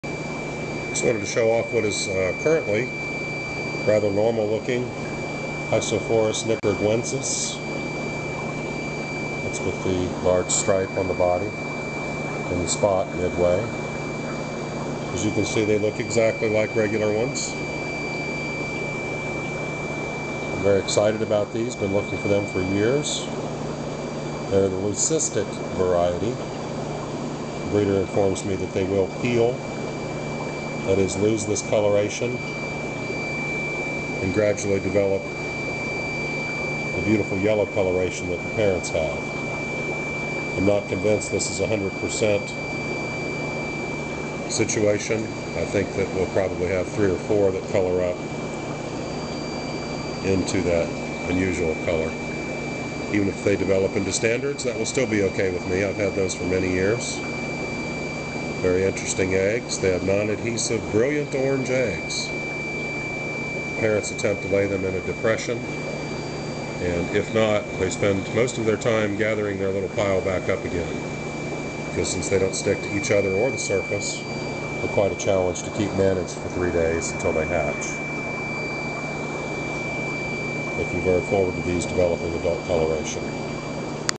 Fishroom Hypsophrys Nicaraquinsis, the Leucistic ones!
Hypsophrys Nicaraguinsis, group of 6 growing nicely 2 1/2 to 3" at time of video.  This is the Xanthic variation that displays brilliant yellow as adults!  I've looked for these for a long time and met the breeder in TX at the FOTAS meeting.  I can't wait for these to develop their yellow color!